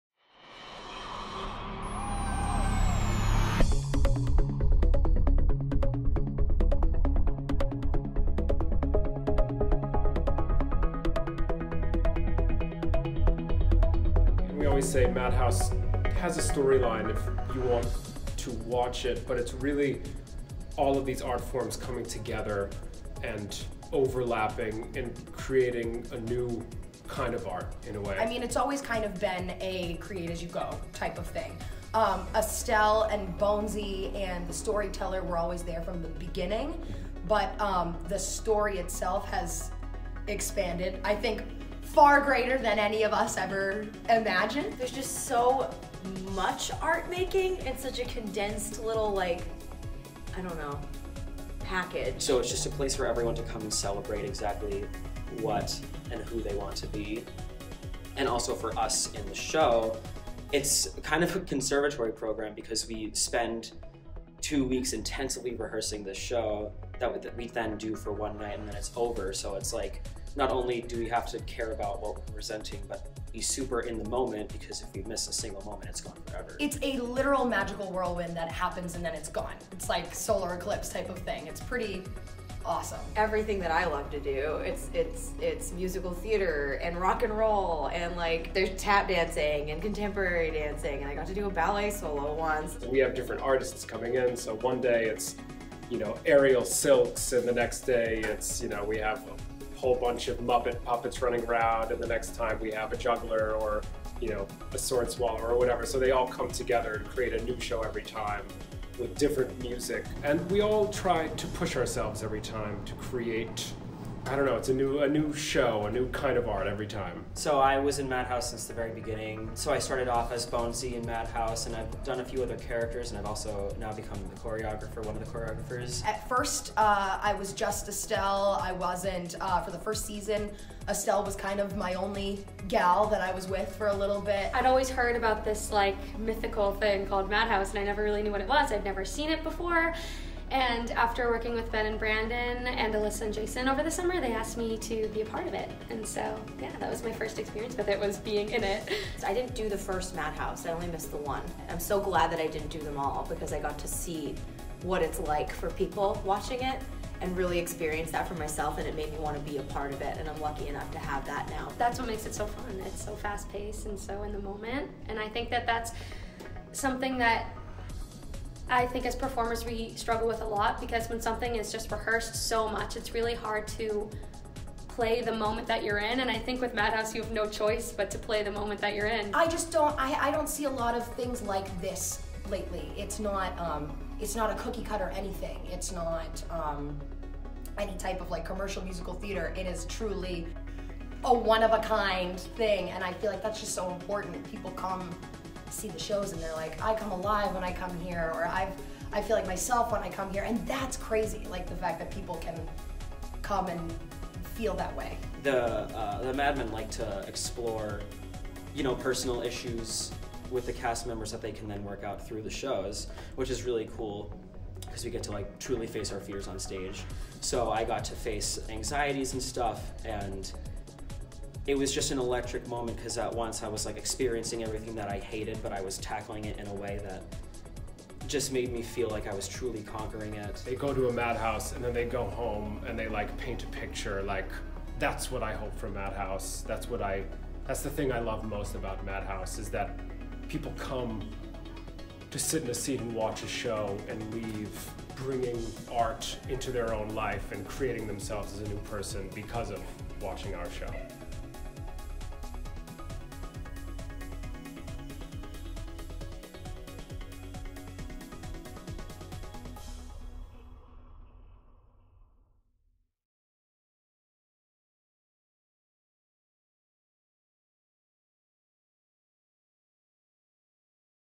Behind the Curtain, March 2019 - 'Mad Haus' at Seacoast Repertory Theatre
Since 2017, 'Mad Haus' has been the artistic playground for dozens of professional artists to flex their creative muscles on stage at the Seacoast Repertory Theatre in Portsmouth, NH as part of their 'Red Light Series'.

Each production runs for one night only incorporating song, dance, elements of circus, and much more.

If you have a project that you think we should highlight in our next look "Behind the Curtain", you can learn more about this initiative and submit a nomination

Includes promotional video content created by Ben Bagley

Produced by Gina Carballo & Justin Voshell on behalf of